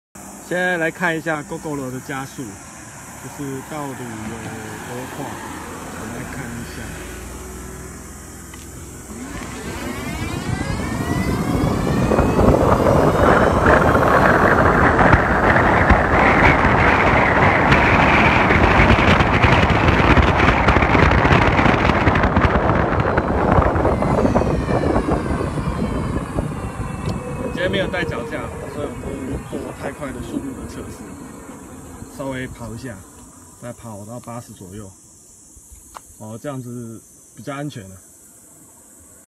GOGORO的0~80km加速
臨時想到，所以拿相機隨便拍，下次用腳架看看，可以拍更高的速度